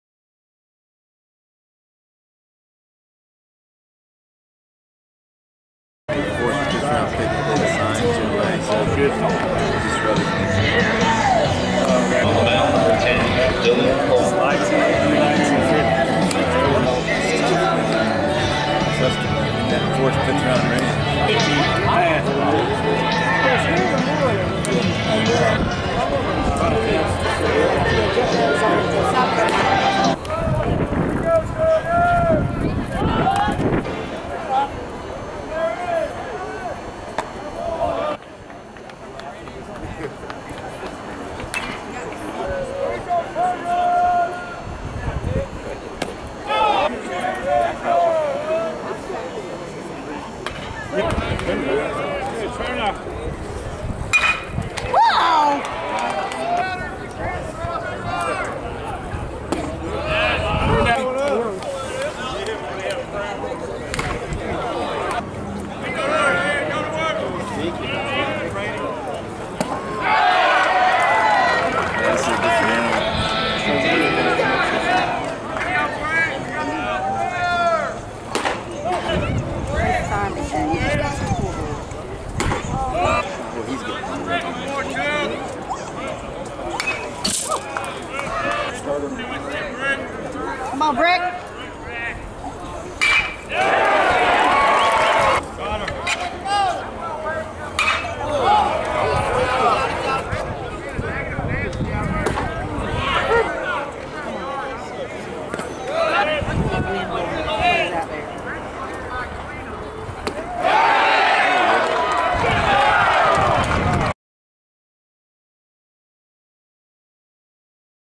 Dylan Coleman — RHP, Missouri State
Missouri State RHP Dylan Coleman pitches against East Carolina on March 3, 2018